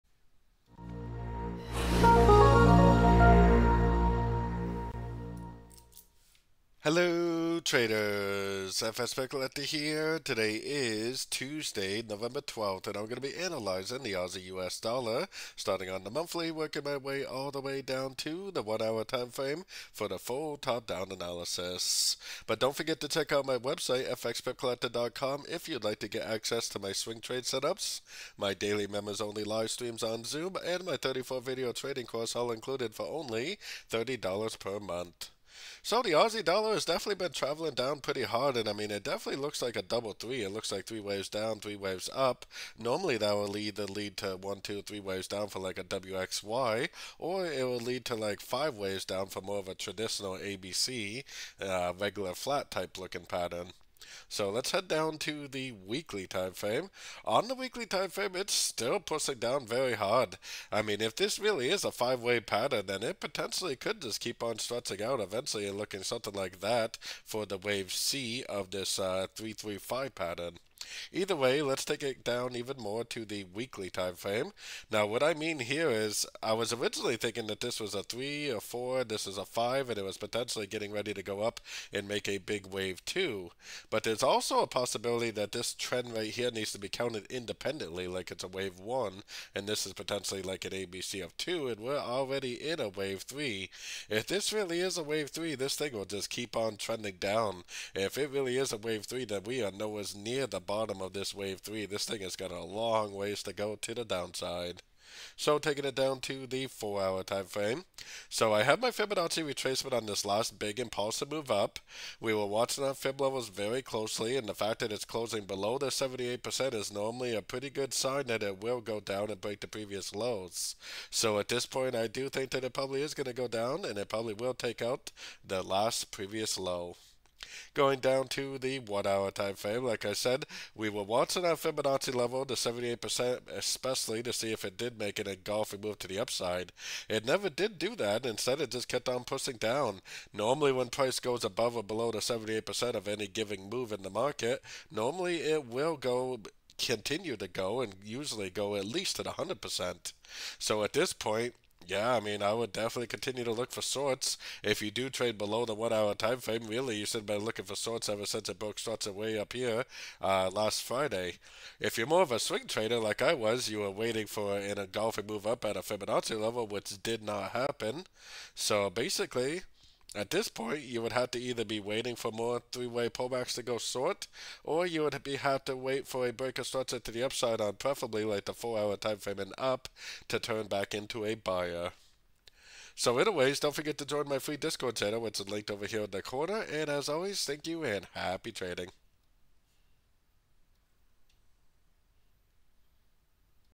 Latest AUDUSD Forecast and Technical Analysis for November 12, 2024
Hello Traders! Here is today's AUDUSD technical analysis. I will be reviewing the AUD/USD forecast using Elliot Wave Theory and Fibonacci. Learn and earn at the same time by seeing all my swing trade entries at the time I take them.

Learn to read the market without indicators and utilize proper risk management when analyzing Forex pairs like AUD to USD.

This video is about Analyzing the Latest Trading Tips For AUD/USD: Technical Analysis for November 12, 2024.